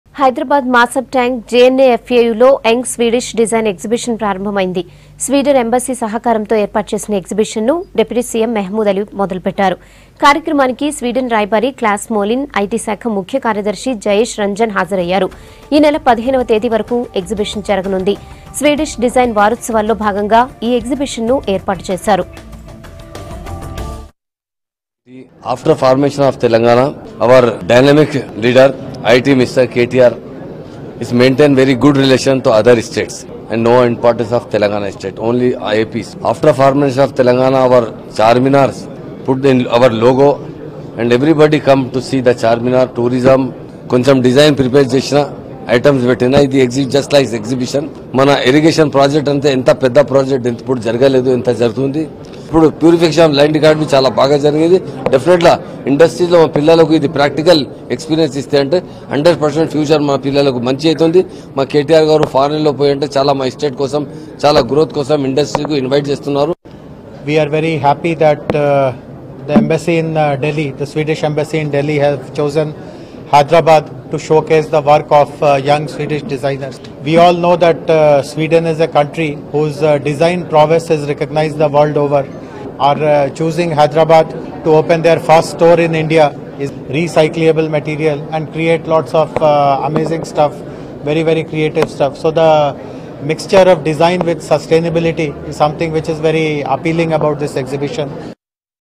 Deputy CM Mahmood Ali Inaugurates Young Swedish Design Exhibition At JNAFAU | Hyderabad | V6 News
Deputy CM Mahmood Ali Inaugurates Young Swedish Design Exhibition At JNAFAU, Hyderabad.